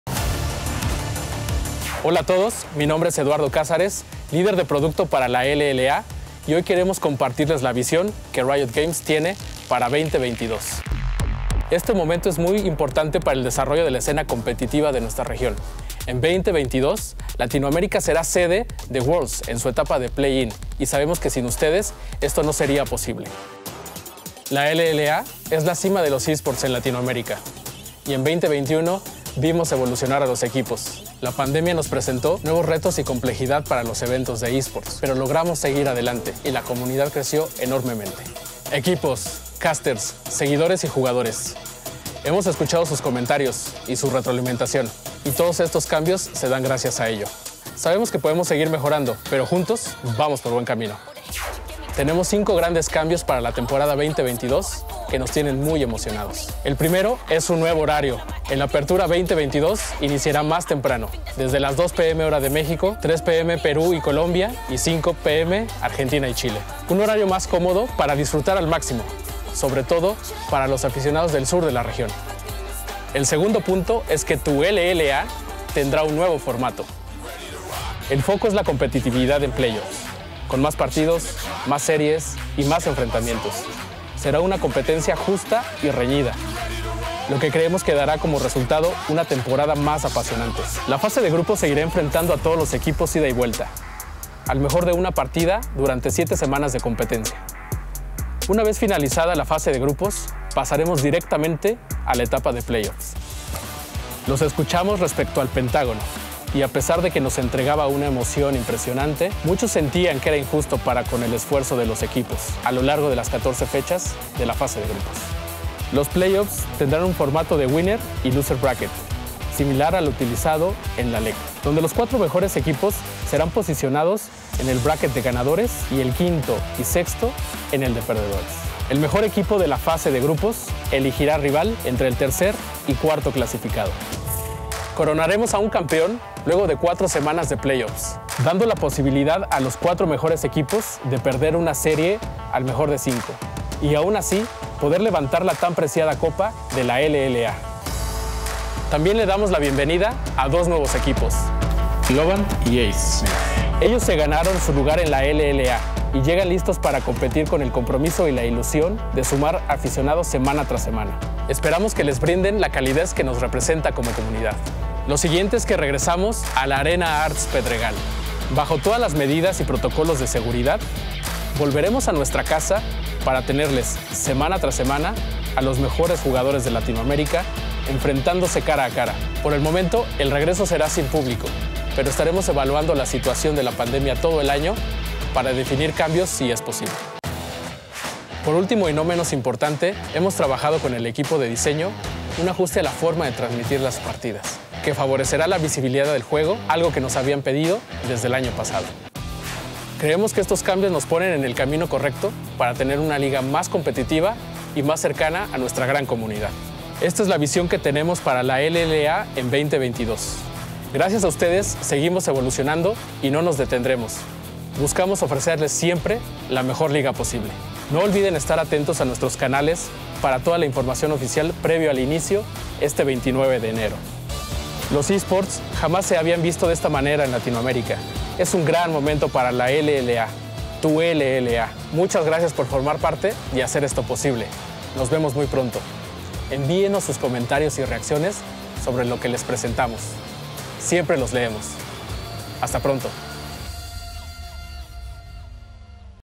Visión LLA 2022 | Esports | League of Legends
¿Tienes dudas sobre el inicio de la temporada 2022 de tu LLA?
Te contamos lo más relevante antes del arranque: nuevo formato, horarios y muchos más detalles que no querrás perderte.